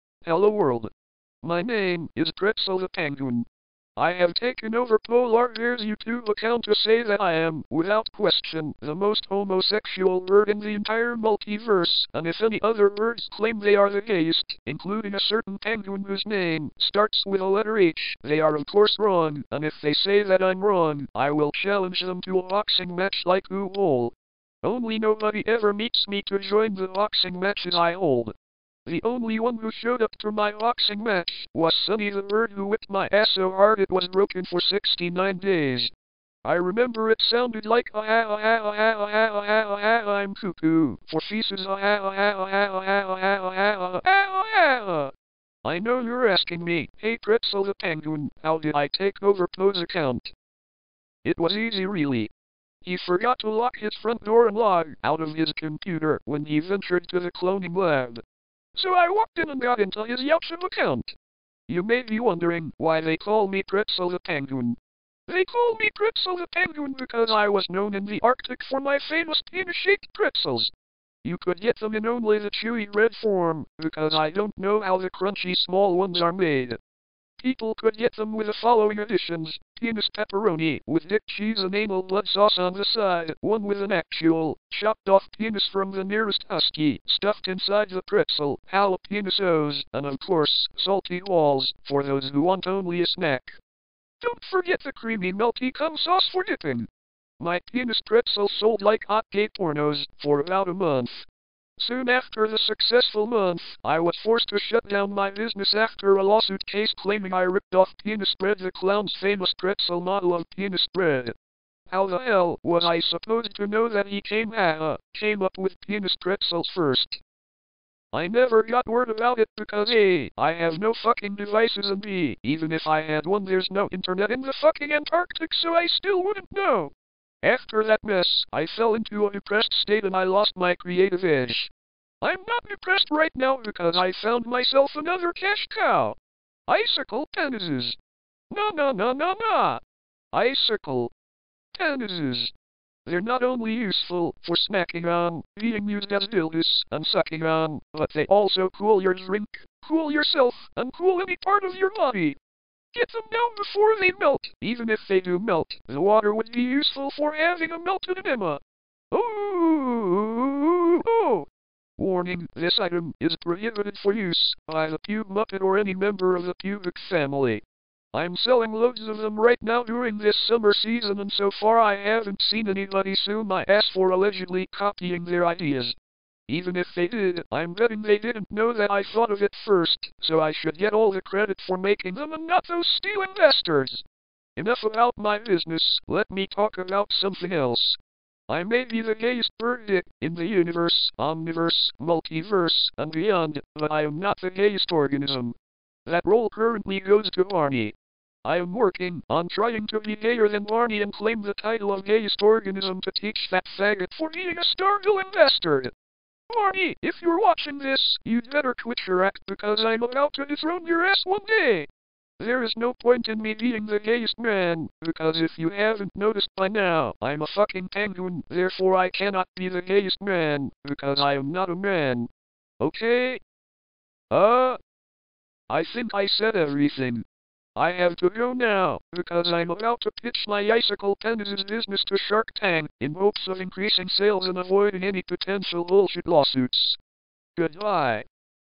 Pretzel the Penguin Talks About Stuff I Guess
Pretzel the Penguin seems to have taken over @PoeLarBearYT 's channel.

This is what he had to say.